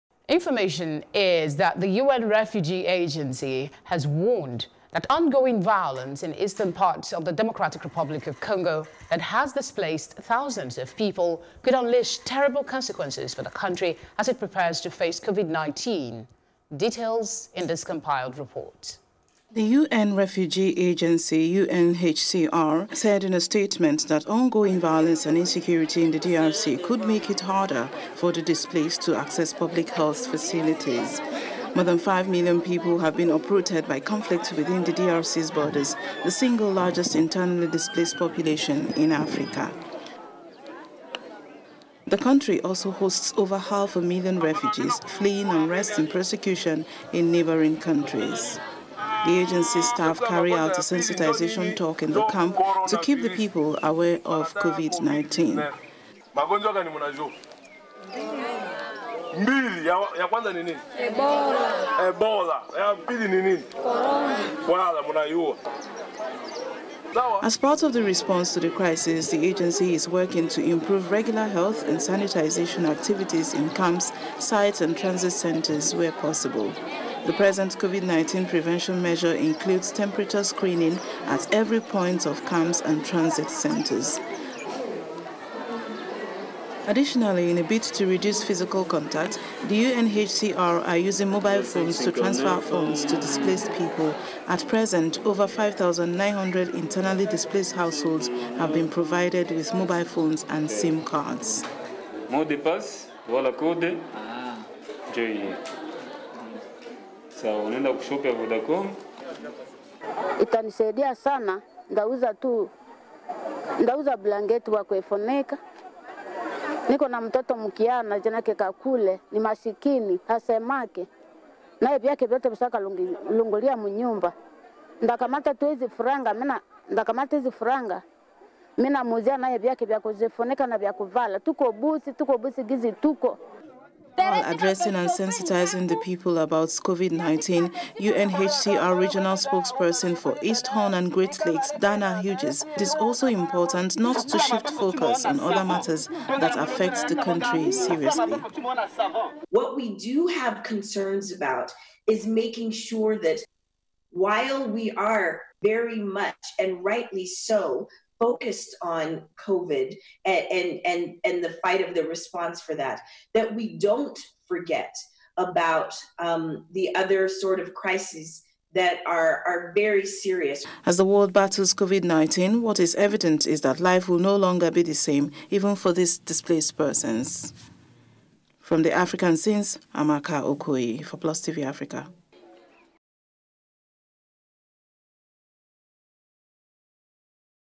COVID-19 Confirmed In The Democratic Republic
The UN Refugee Agency (UNHCR) has warned that ongoing violence in eastern parts of the Democratic Republic of the Congo (DRC) that has displaced thousands of people could unleash terrible consequences for the country as it prepares to face COVID-19.